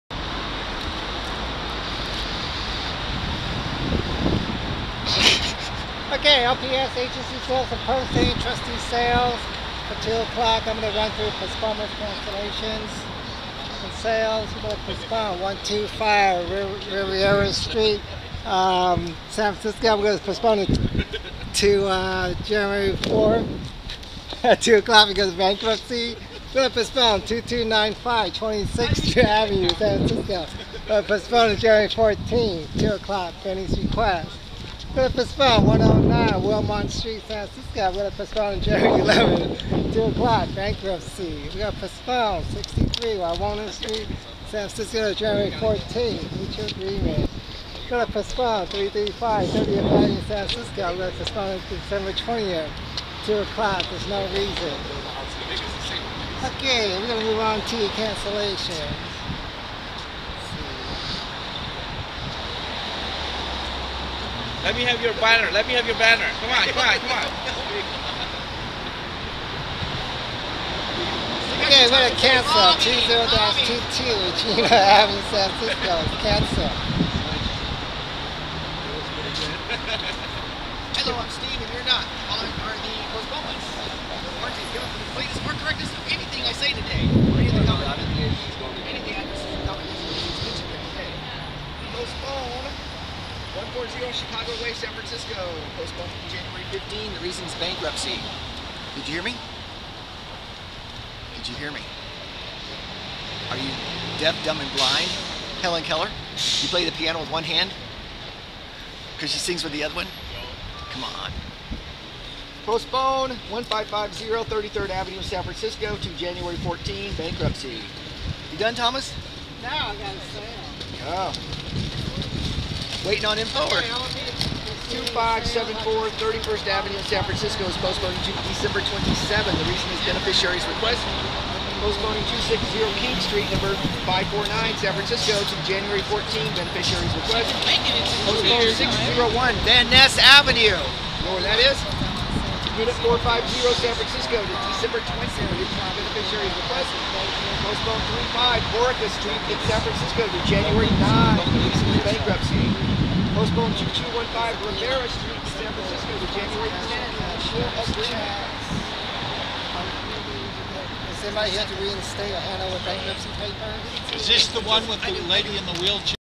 Part II of Occupy the Auctions on December 13, 2012, in San Francisco
Part II of Occupy the Auctions on December 13, 2012, in San Francisco (VID00002.MOV)